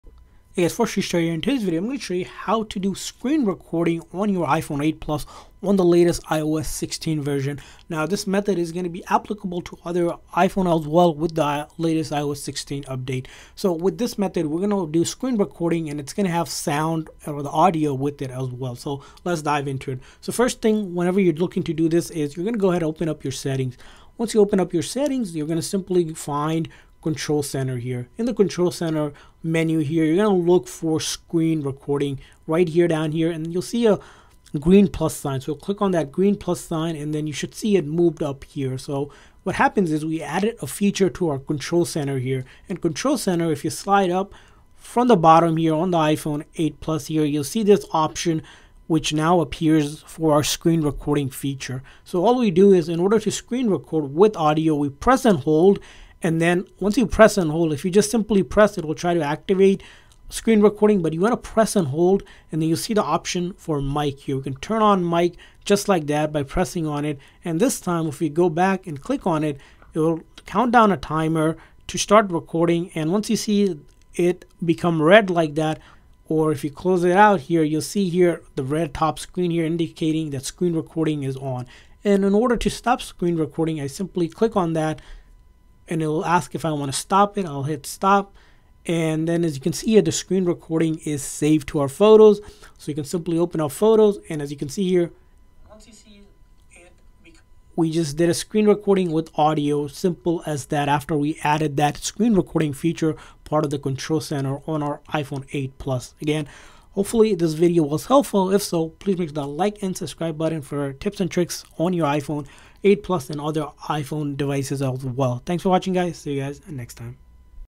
iPhone 8 Plus: How to Screen Record with Audio Sound
In this comprehensive video, we'll guide you through the process of screen recording with audio on your iPhone 8 Plus. Whether you want to capture a gameplay session, create a tutorial, or save a video clip with its accompanying audio, screen recording is an incredibly useful feature.

Our expert instructions will demonstrate the exact steps you need to take to enable screen recording with audio sound on your iPhone 8 Plus. We'll walk you through the necessary settings and options, ensuring that you can capture both the visual and audio components seamlessly.

By following our step-by-step guide, you'll be able to record your screen activities with crystal-clear audio sound, allowing you to create engaging content or keep a record of important moments. We'll also provide tips and tricks to optimize your screen recording experience.

We regularly update our content to keep you informed about the latest features and functionalities of your iPhone.